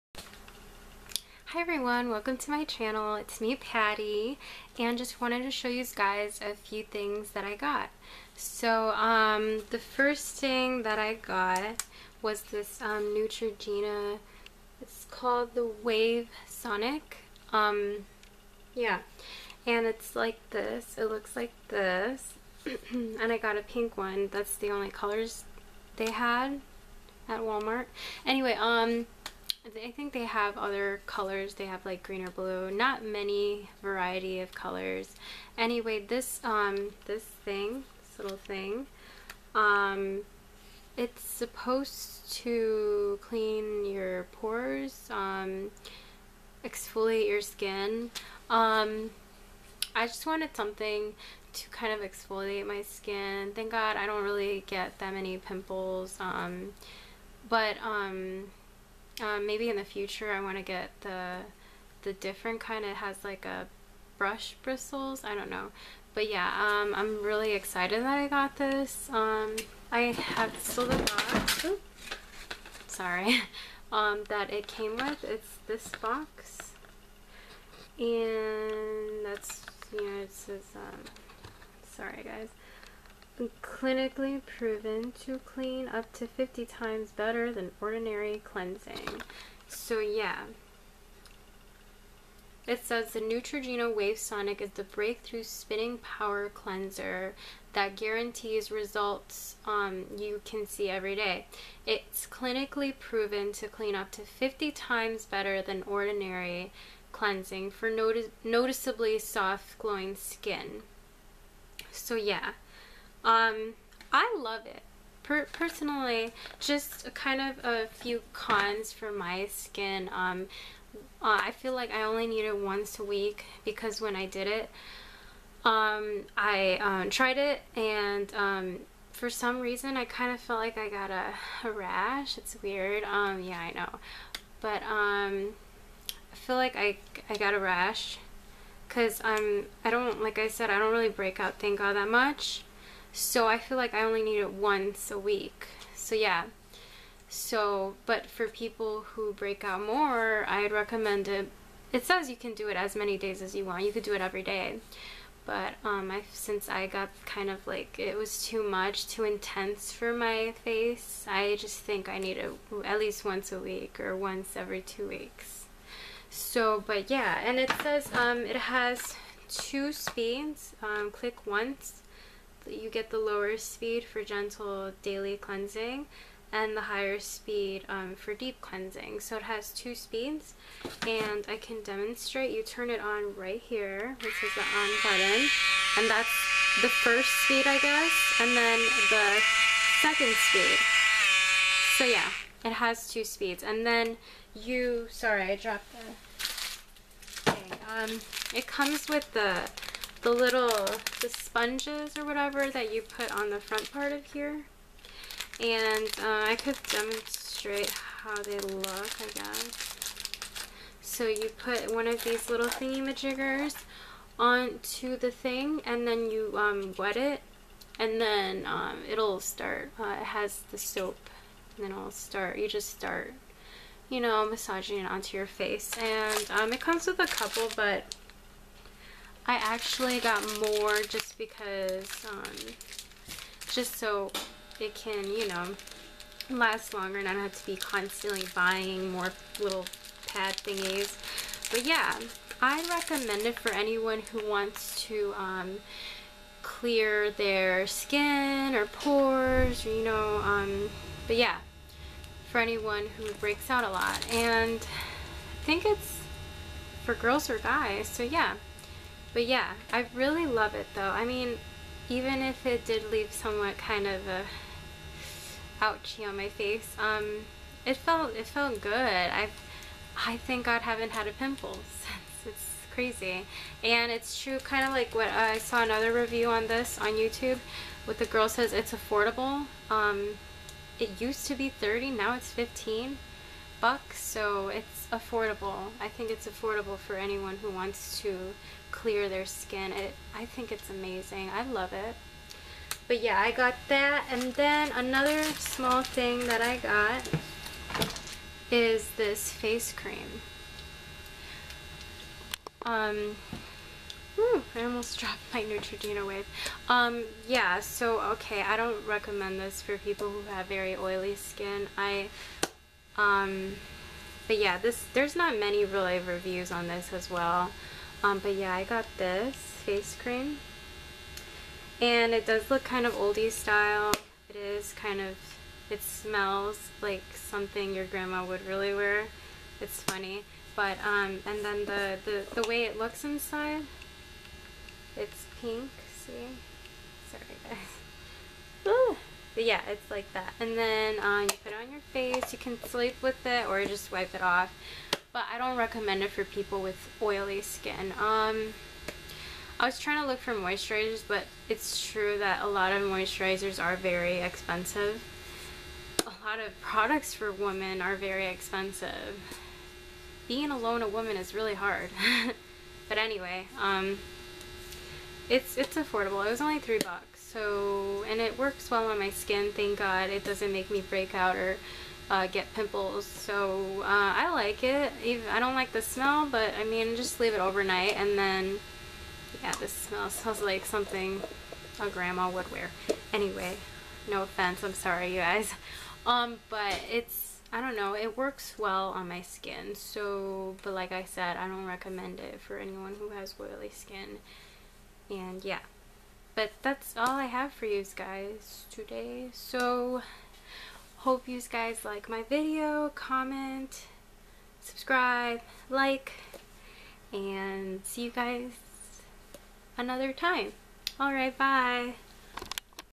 Neutrogena Wave Review
Also got the Jergen's Face cream but that is not commonly used by many girls to this day only a few people use it like I mention in my video! But yea hope you's enjoy my video :)